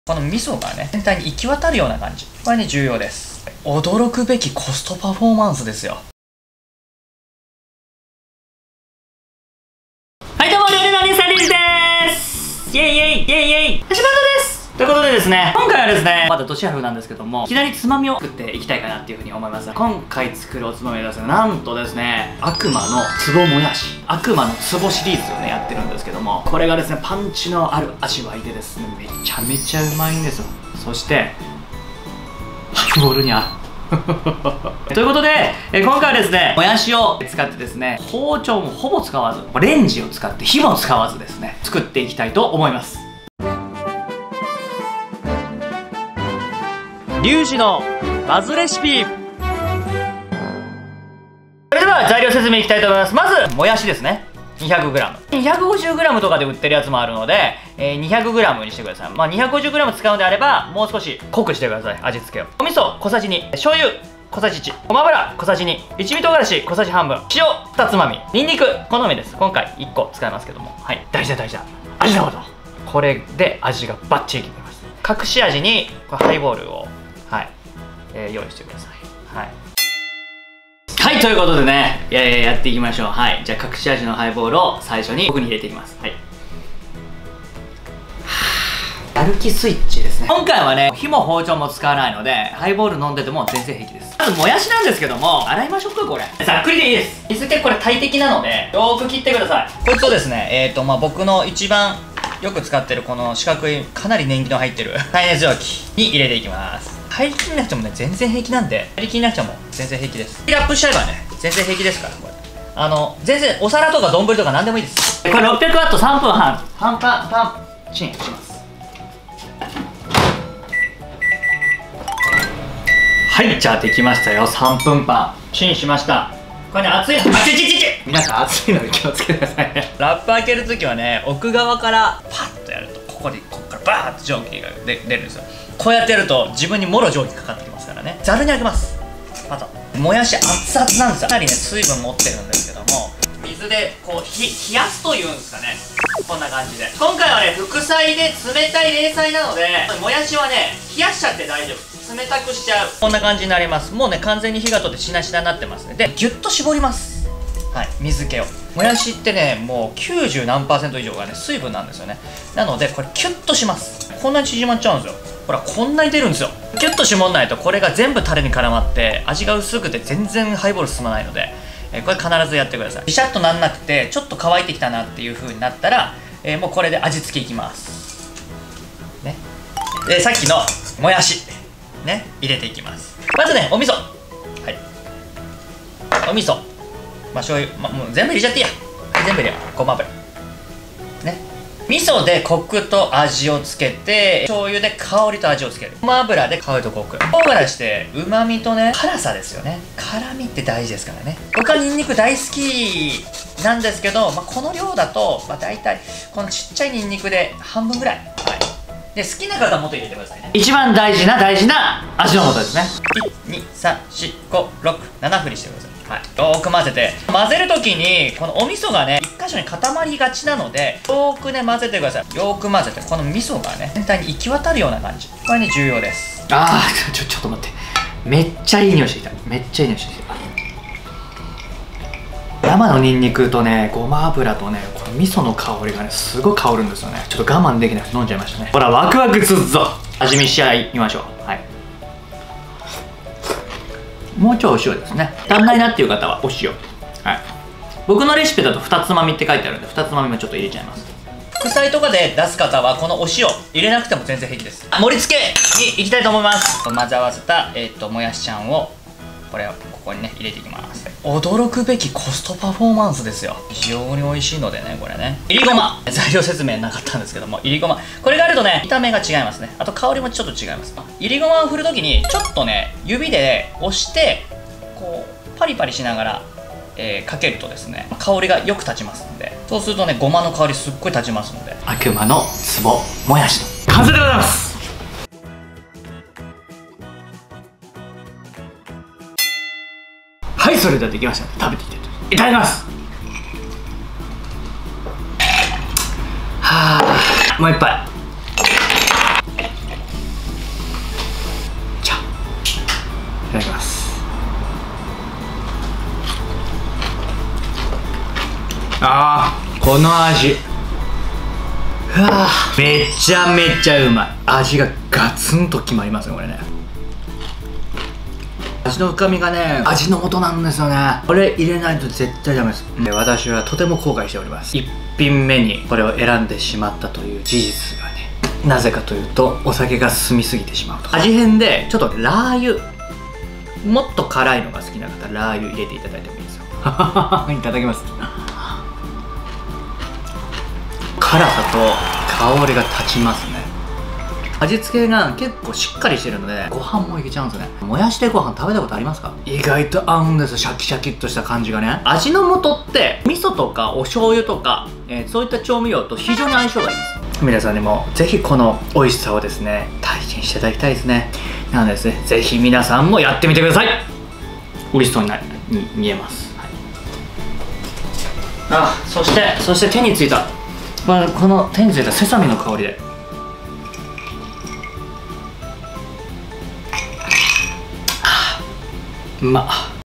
これだけで米2合食えます。ずっと壺に保存しておきたいほど美味しいもやしです【悪魔の壺もやし】
【悪魔の壺もやし】

今回は味の素さんとのコラボになります！
簡単おいしいメニューはこちらでも紹介しています！

★今回のレシピはこちら↓
【悪魔の壺もやし】（1~2人前）
もやし　　　200g
味噌　　　　小さじ2
醤油　　　　小さじ1
塩　　　　　ふたつまみ
『味の素』　7振り
一味唐辛子　小さじ1/2
ごま油　　　小さじ2
にんにく　　少々
いりごま　　適量
★お好みで　ラー油

フリーBGM
DOVA-SYNDROME